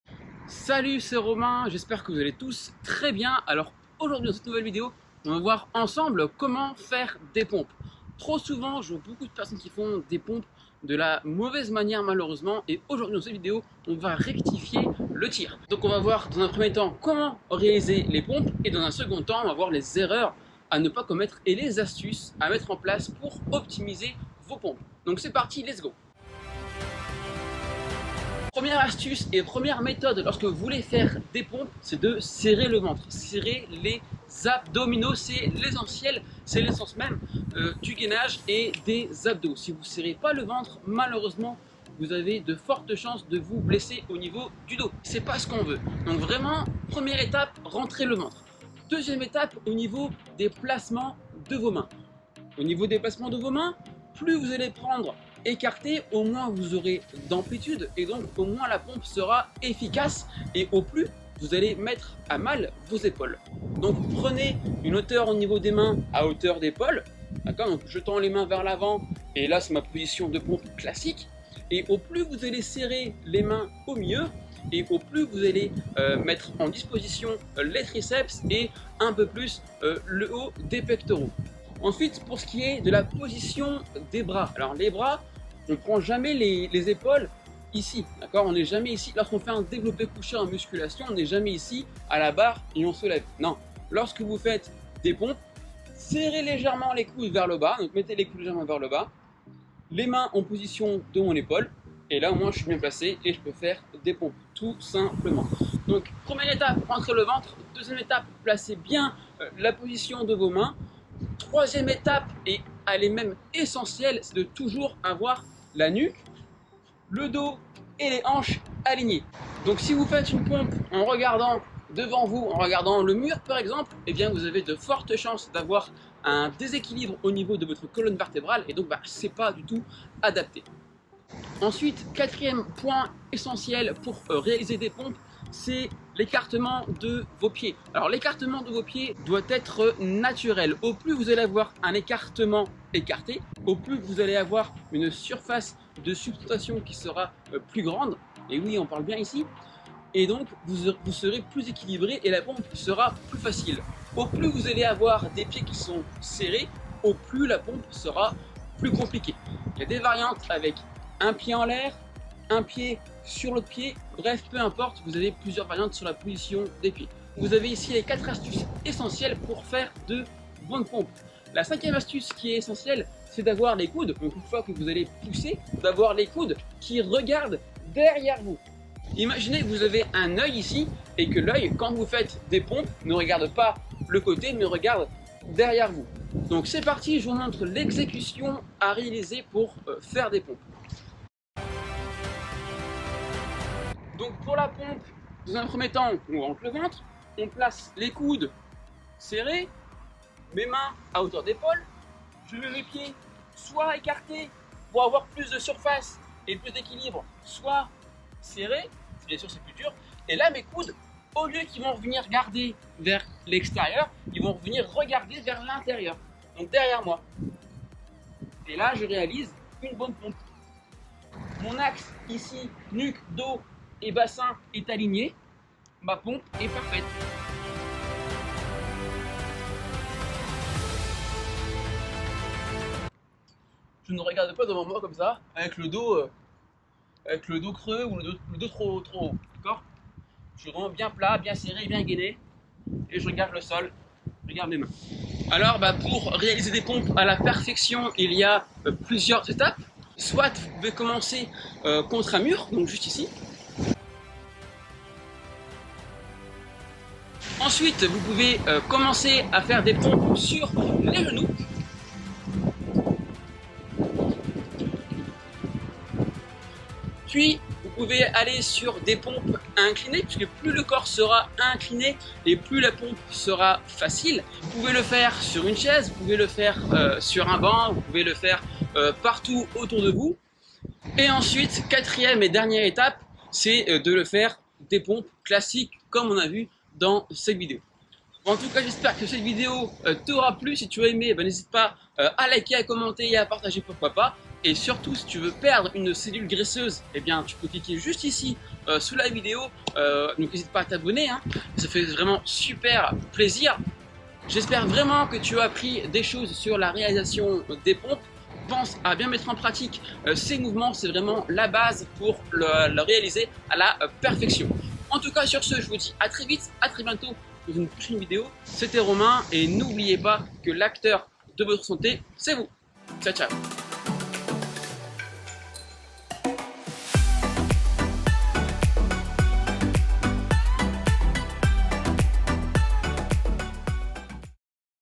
Comment faire des pompes
Comment faire des pompes correctement pour éviter toutes les erreurs. Réaliser des pompes à la perfections ! De débutant à expert.
▬▬▬▬▬▬▬▬▬ GUIDE OFFERT▬▬▬▬▬▬▬▬▬
Recevez gratuitement 1H de vidéo sur la nutrition et un challenge de 5 jours pour atteindre votre objectif.

▬▬▬▬▬▬▬▬▬    Infos  ▬▬▬▬▬▬▬▬▬
Beaucoup de personnes réalise des pompes pour se muscler. Hélas très souvent cette réalisation d'exercice et parfois mal exécuté, avec un ventre gonflé, un positionnement des bras non adapté. Bref une multitude de facteur qui à long terme nuis sur votre amélioration.

C'est pourquoi à travers cette vidéo, je vous partages mes conseils pour réaliser des pompes facilement et ceux ; même quand on est débutant.

Les pompes restent un exerce de base. Il sera dommage d'avoir une mauvaise exécution sur ce mouvement. Qui est pourtant redoutable pour venir travailler l'ensemble de votre corps et notamment le Haut du corps !

Les pompes restent pour moi l'un des meilleurs exercices au poids de corps pour travailler les muscles du triceps et surtout du pectoraux.

Romain Passanante